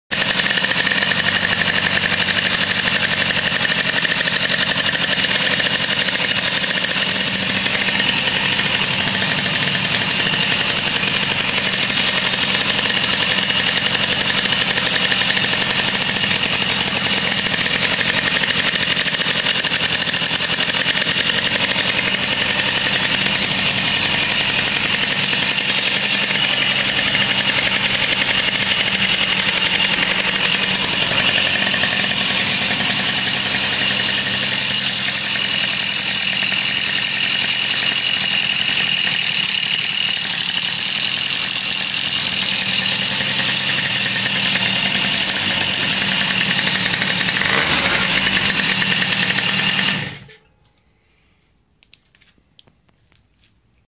Polini 70cc start up
start up after honing a scratch out of the cylinder a new ring, lowered compression ratio.